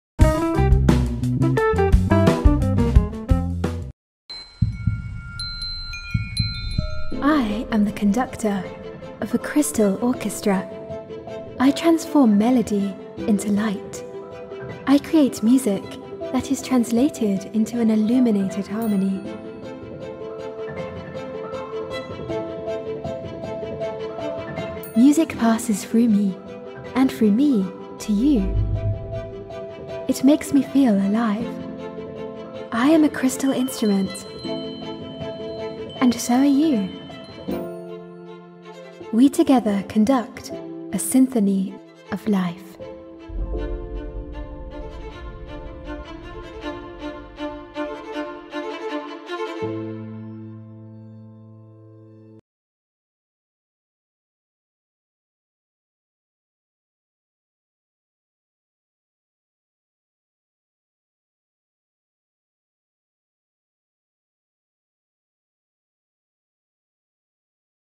PRECIOSA LAUNCHES “COMPOSITION IN CRYSTAL” AT MILAN DESIGN WEEK
Light, crystal & music, renowned Preciosa launches ”Composition in Crystal”, an interactive installation at Milan Design Week in Tortona from 7-12 June, 2022.

Video Courtesy of Preciosa